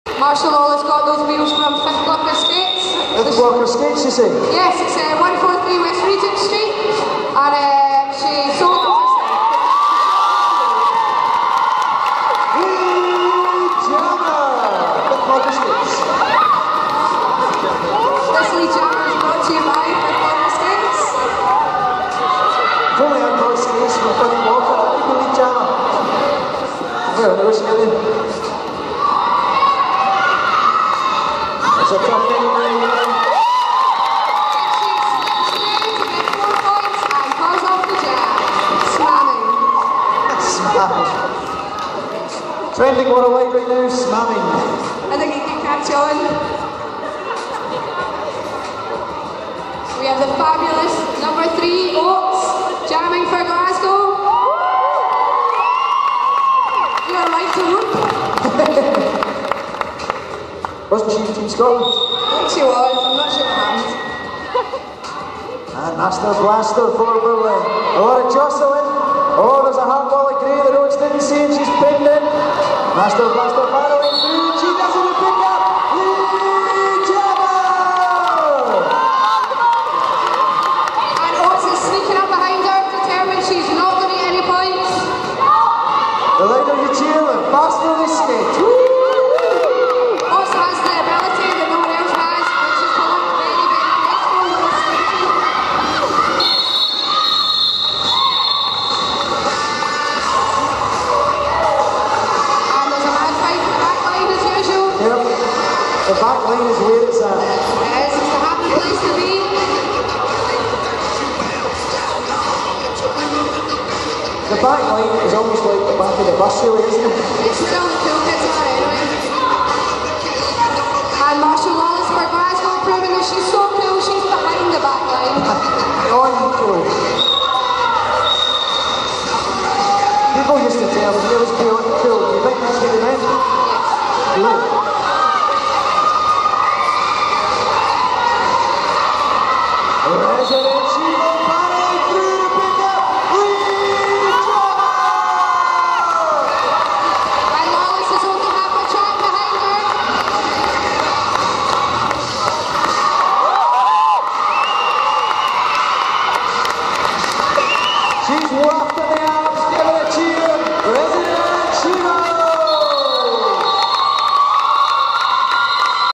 GRD Irn Bruisers v BCRD Berlin Bombshells 10 March 2012 Jam22,23,24 FINAL
Glasgow Roller Derby "Irn Bruisers" v Bear City Roller Derby "Berlin Bombshells"
10 March 2012 @ ARC, Glasgow Caledonian University

This is the 22nd, 23rd and 24th jams of the 2nd Period; the antepenultimate, penultimate and final jams of the bout.

Your jammers are: Marshall Lawless (42) and Oates (3) for Glasgow Roller Derby
Candy Darling (19), Master Blaster (2) and Resident Shevil (28) for Bear City Roller Derby

Your announcers are: Helliverance and Haberbashery

All footage captured on a Sony A580 with a Minolta 34-105mm lens.